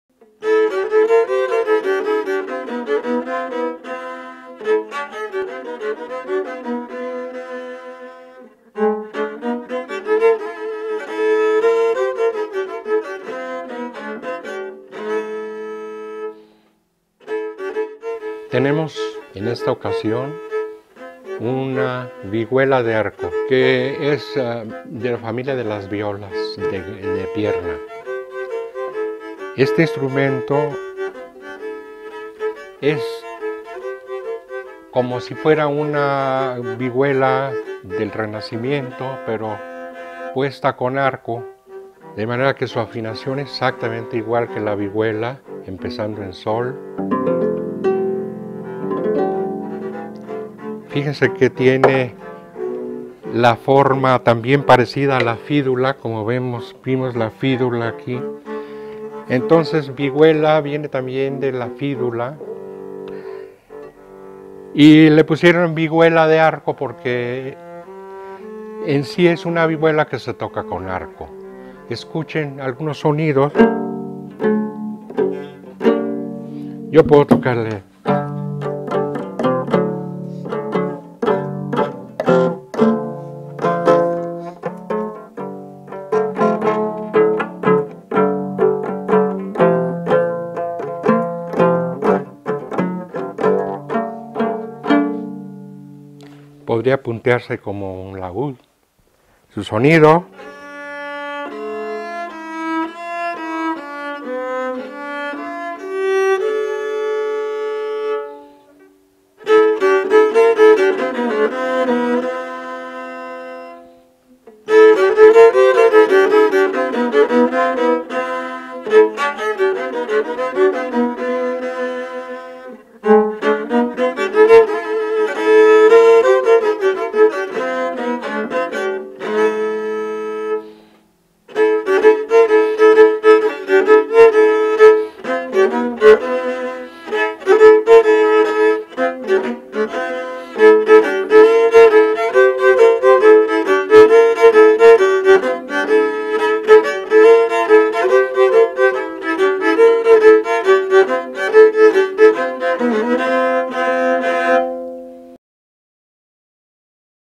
¿Cómo suena la Vihuela de Arco? | Música Antigua | Universidad de Guanajuato
La viola de arco, vihuela de arco, fídula, viela o giga es un instrumento de cuerda frotada similar al violín, aunque con un cuerpo algo más largo y más profundo, de tres a cinco cuerdas y un clavijero. El sonido se obtiene al frotar las cuerdas con un arco, teniendo el instrumento apoyado en el pecho o en el muslo. Alcanzó gran popularidad durante la Edad Media, siendo utilizado por los trovadores y los juglares desde el siglo XIII al siglo XV.